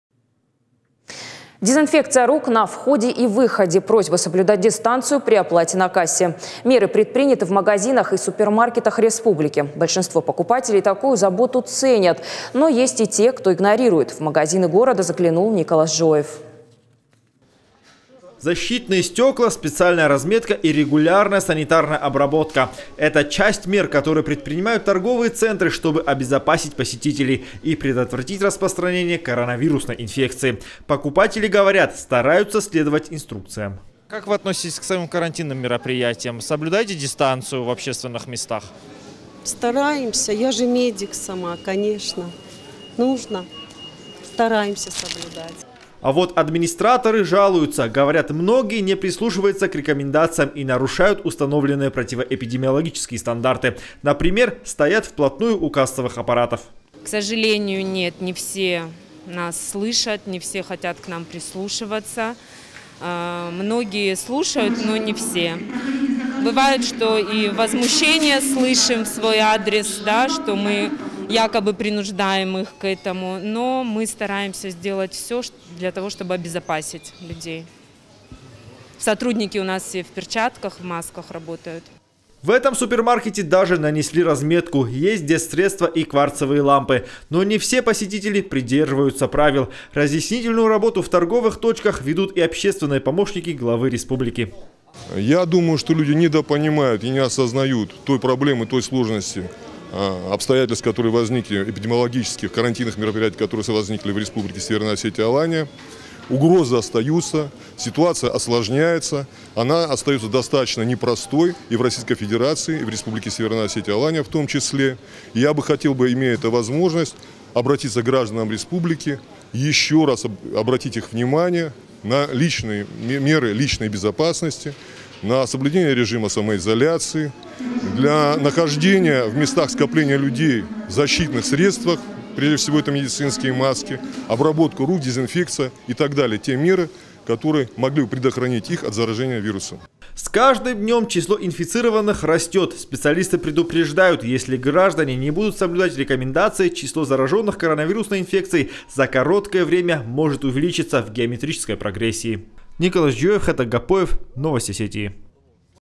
Общественные помощники главы республики проверили работу супермаркетов в условиях пандемии COVID19
Дезинфекция рук на входе и выходе, просьба соблюдать дистанцию при оплате на кассе. Меры предприняты в магазинах и супермаркетах республики. Большинство покупателей такую заботу ценят, но есть и те, кто игнорирует.

Защитные стёкла, специальная разметка и регулярная санитарная обработка - это часть мер, которые предпринимают торговые центры, чтобы обезопасить посетителей и предотвратить распространение коронавирусной инфекции. Покупатели говорят - стараются следовать инструкциям.

А вот администраторы жалуются. Говорят, многие не прислушиваются к рекомендациям и нарушают установленные противоэпидемиологические стандарты. Например, стоят в плотную у кассовых аппаратов.

В супермаркете нанесли специальную разметку, есть дезинфицирующее средство и кварцевые лампы, но не все посетители придерживаются правил. Разъяснительную работу в торговых точках ведут, и общественные помощники главы республики.

С каждым днём число инфицированных растёт. Специалисты предупреждают, если граждане не будут соблюдать рекомендации, число заражённых коронавирусной инфекцией за короткое время может увеличится в геометрической прогрессии.

Николас Джиоев
Мы рядом в:
ok.ru/irystontv